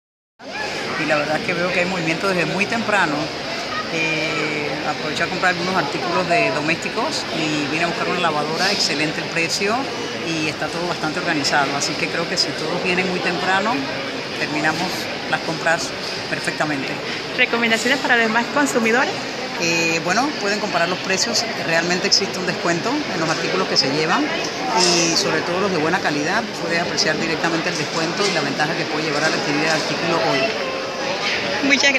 Consumidora satisfecha con sus compras en Albrook Mall
Quienes estuvieron temprano en el centro comercial Albrook Mall encontraron buenas ofertas.